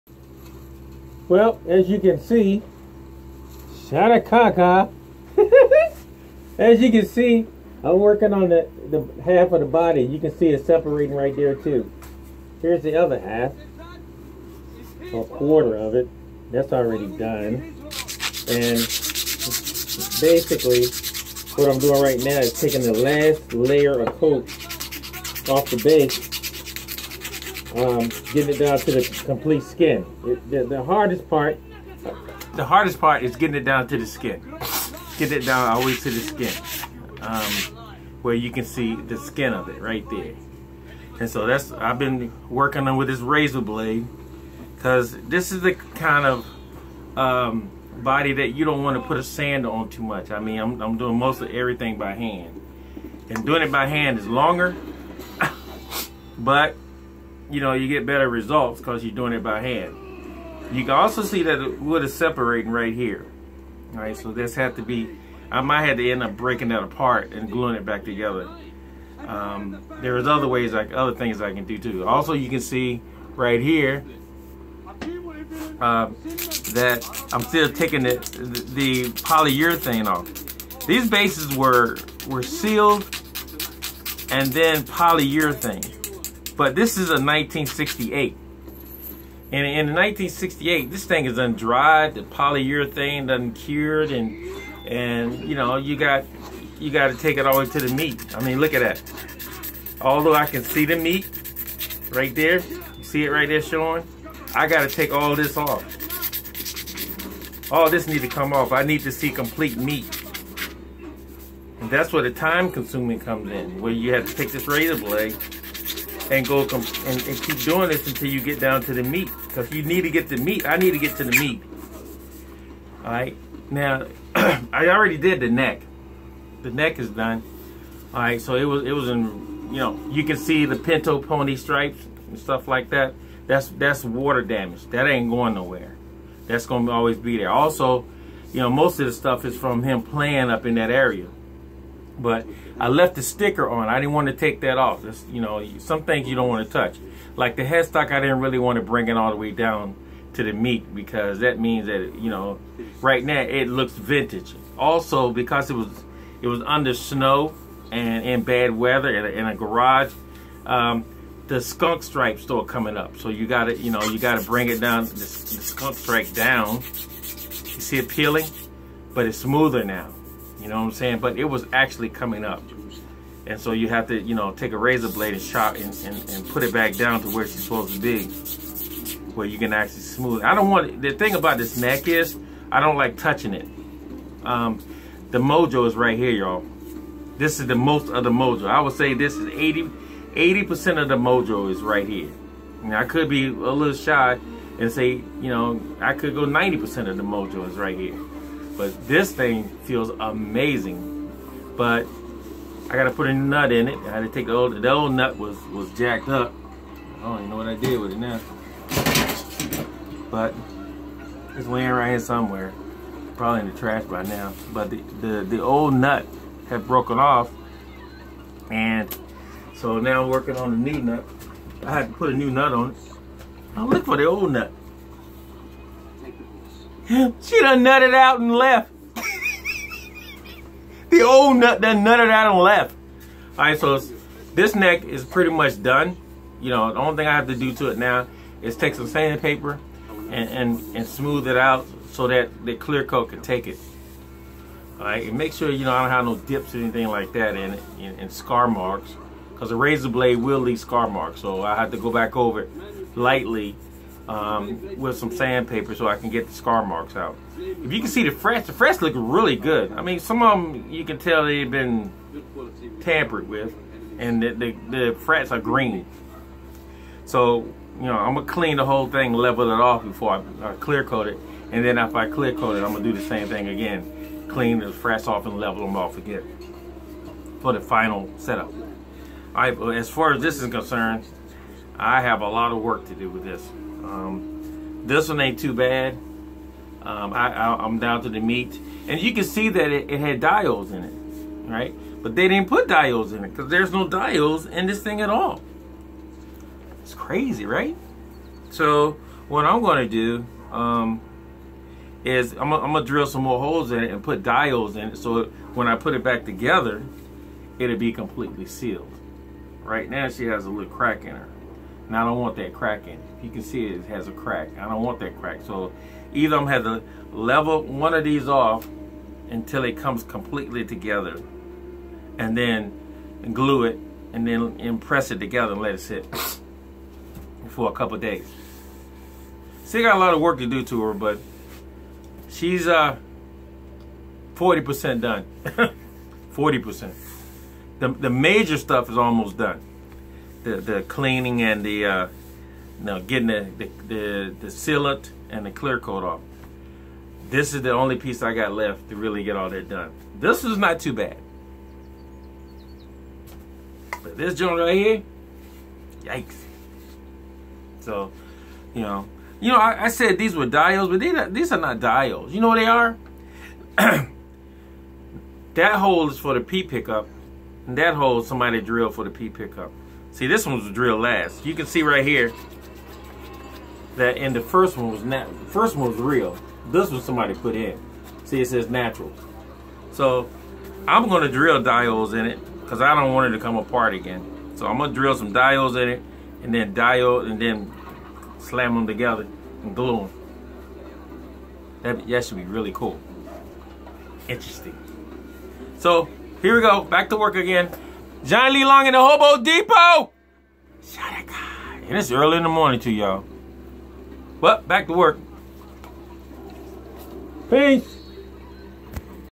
Refinishing 1968 FENDER P BASS CUSTOM
Slow and a lot of hand work,she is old so alot of electrical tools could not be good 4 her..I am now 2 weeks in and still have a long way 2 go...but the end results are amazing